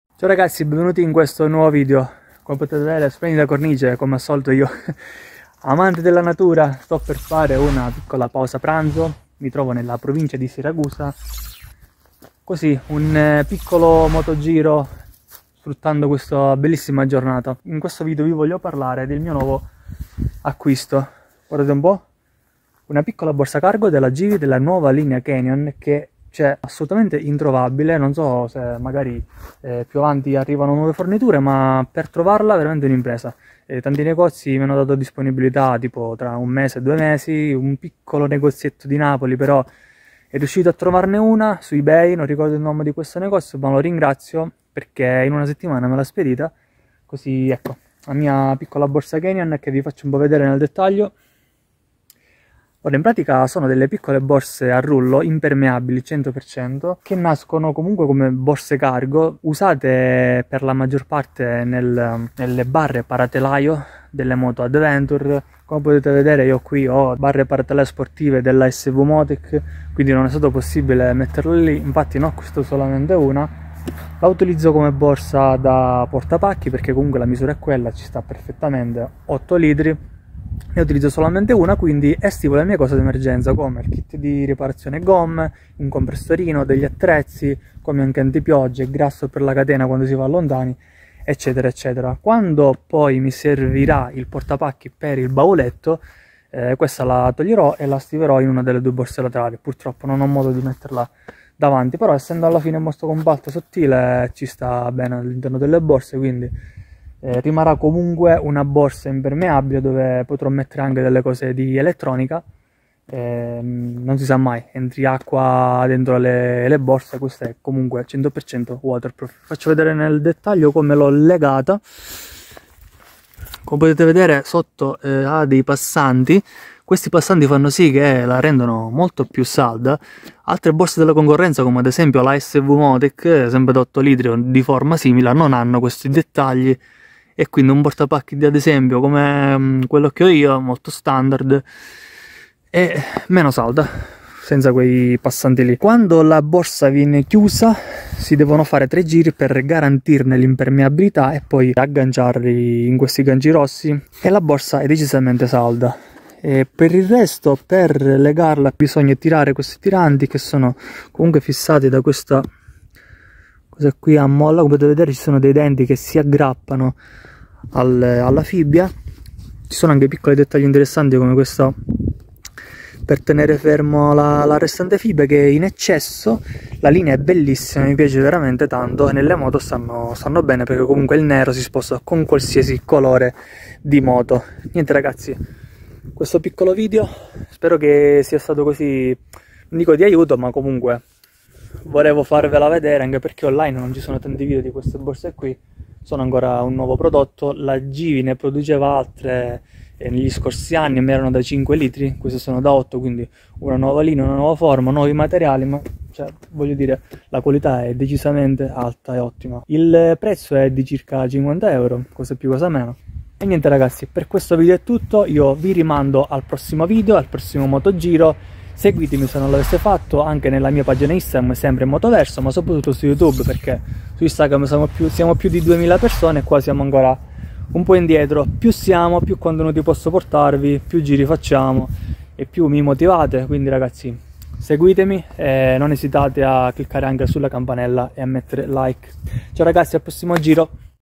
GIVI CANYON GRT722 8 LITRI - OPINIONI
Borsa cargo Givi da 8 litri della linea canyon (codice borsa grt722) molto utile per attrezzi e cose d'emergenza, ottima sia come borsa da portapacchi che da paratelaio.